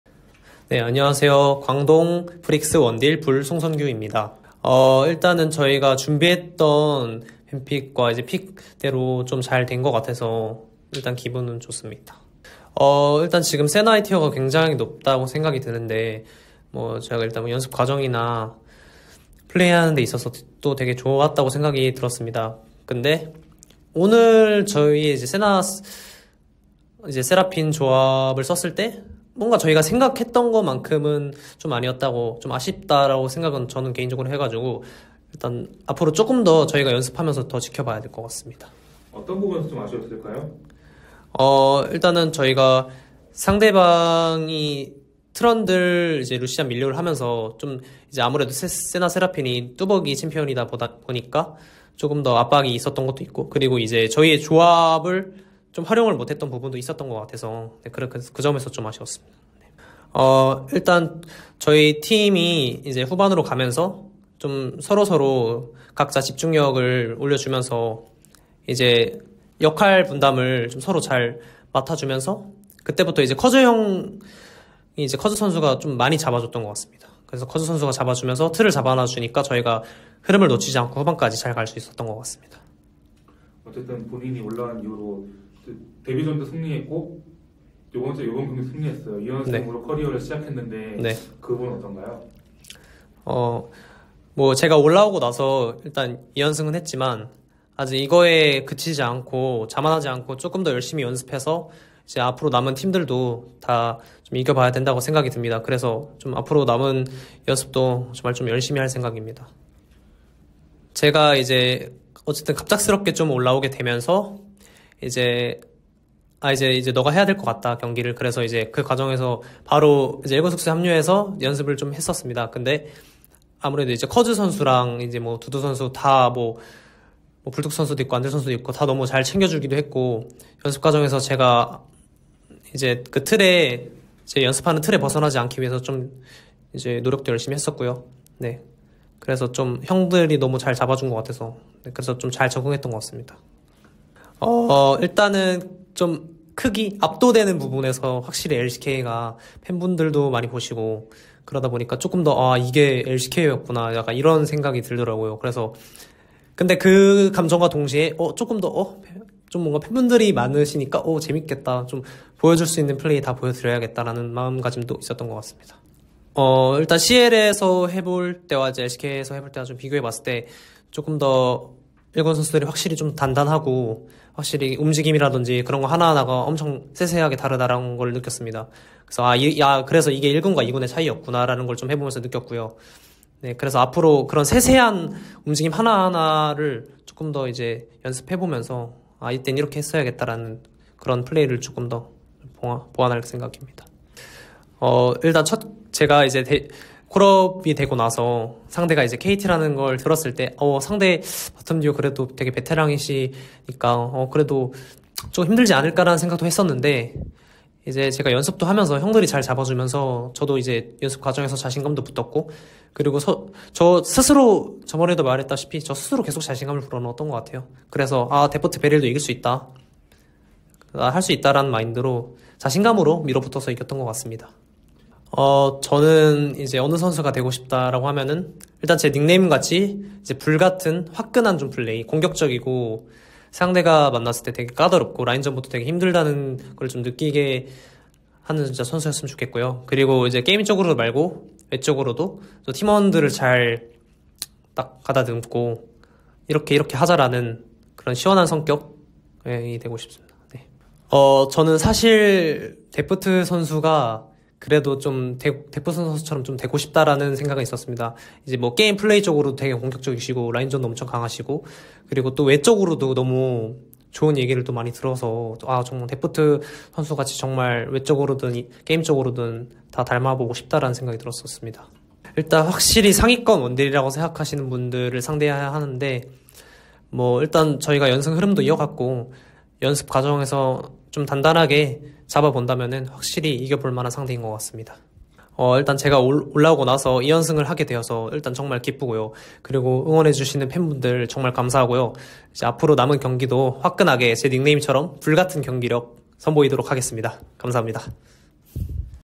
LCK 데뷔 2연승 광동프릭스 불 "롤모델은 데프트, 불같은 공격력의 선수가 되고싶다"
2024년 1월 28일 LCK 경기 승리 선수 인터뷰 광동 프릭스 '불' 송선규